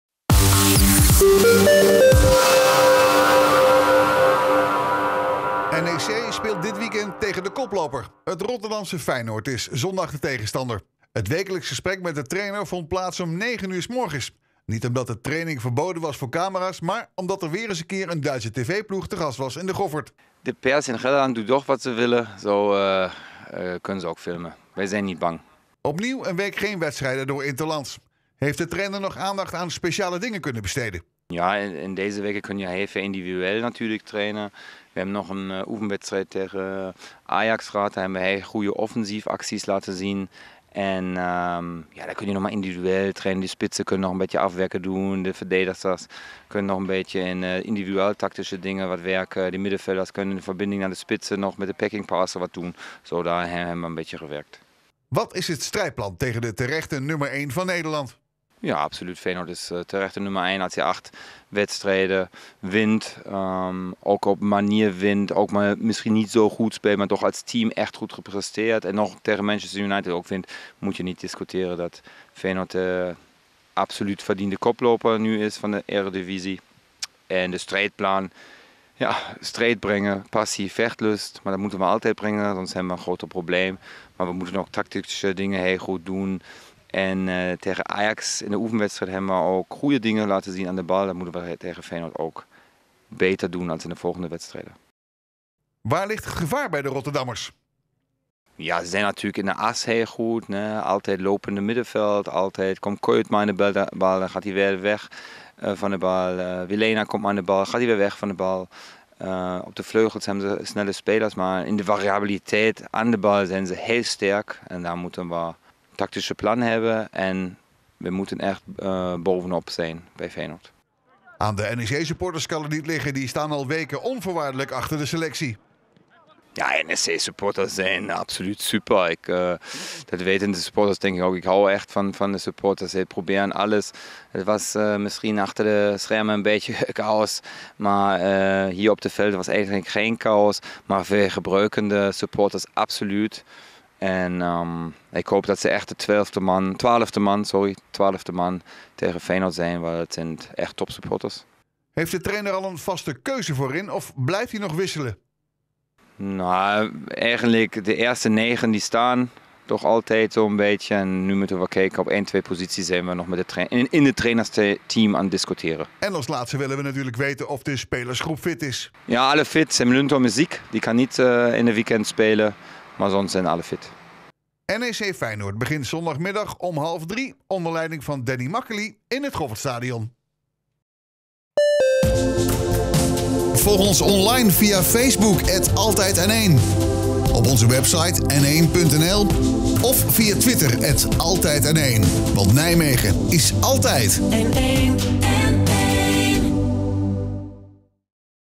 Peter Hyballa (NEC): 'We moeten strijd, passie en vechtlust brengen'
Twee weken na de broodnodige uitoverwinning op bezoek bij concurrent Roda JC betreden de manschappen van trainer Peter Hyballa zondag weer het heilige gras van De Goffert. Lijstaanvoerder Feyenoord komt op bezoek. Gaat NEC de ‘stadionclub’ voor het eerst puntverlies afsnoepen dit seizoen?